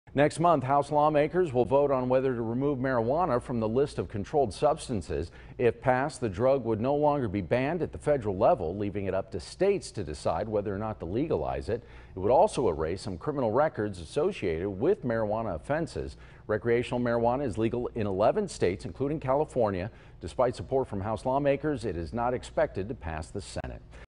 House Will Vote On Taking Cannabis Off Controlled Substances List
The House will vote in September on removing cannabis from the Controlled Substances Act and erasing some marijuana criminal records.